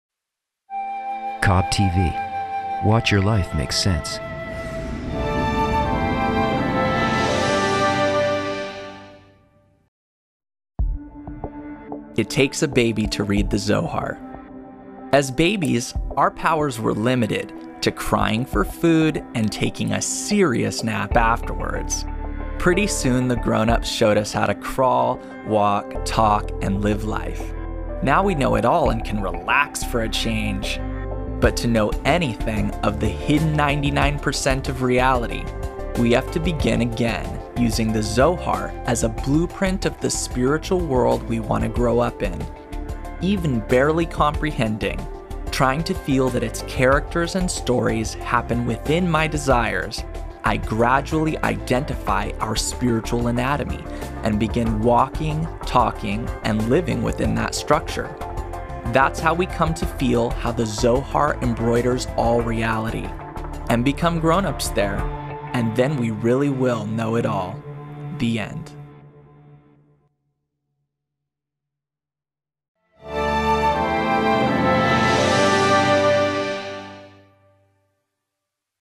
Kabbalah in 60 Seconds. It Takes a Baby To Read The Zohar.
» Sign up for a Free Kabbalah Course today!

For millions of years, the war between hot and cold forces transformed the earth. This battle exists at the human level where the forces are called altruism and egoism. We need to develop a cooling altruistic force to balance the heat of egoism.
Click The Link To Start A Free Kabbalah Course.